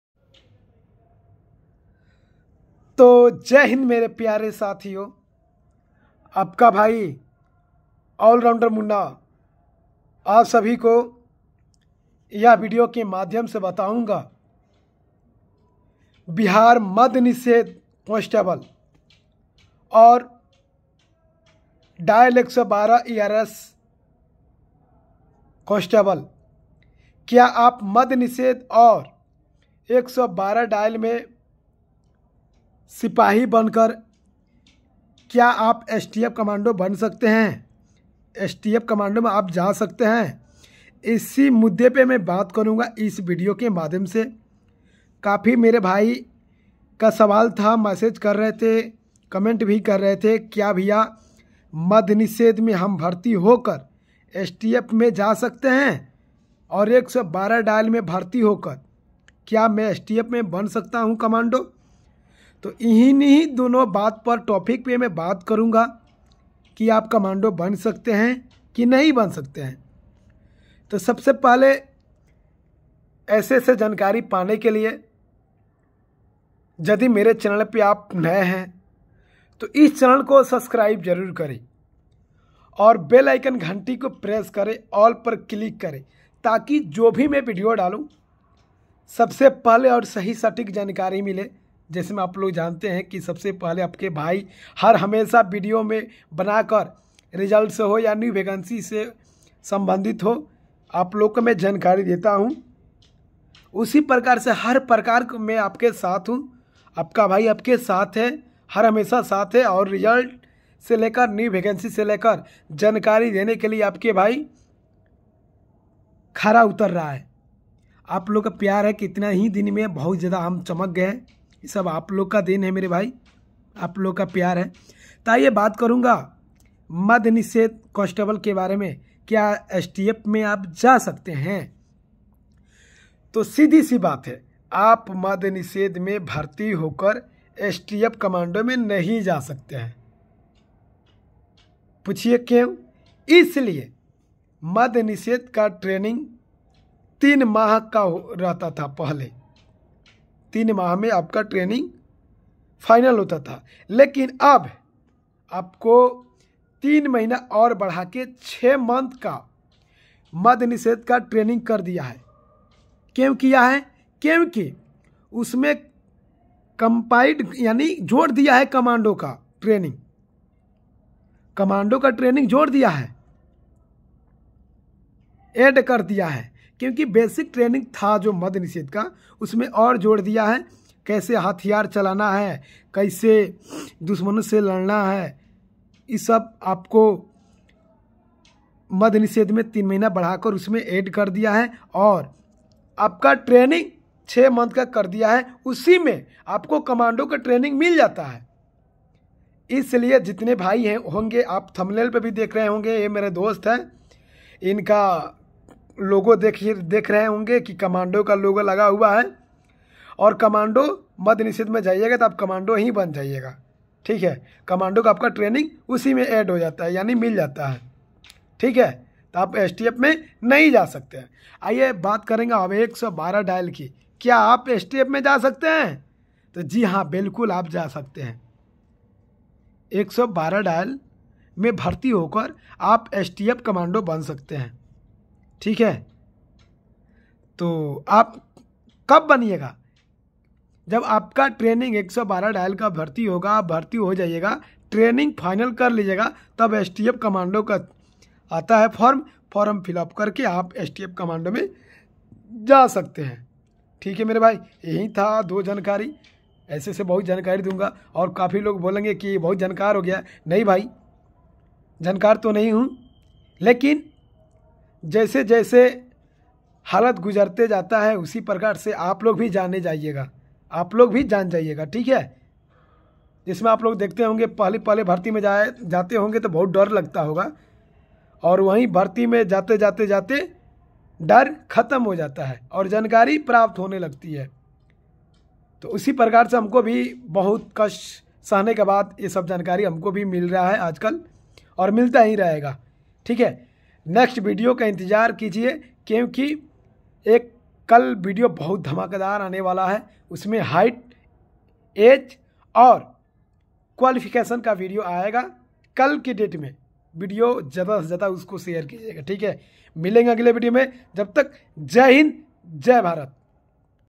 बिहार पुलिस मध निषेध और डायल 112 मे भर्ती होकर ||क्या आप STF COMMANDO मे जा सकते है
वीडियो motivation video bihar police STF COMMANDO kase bane bihar police madh nishedh ERSS 112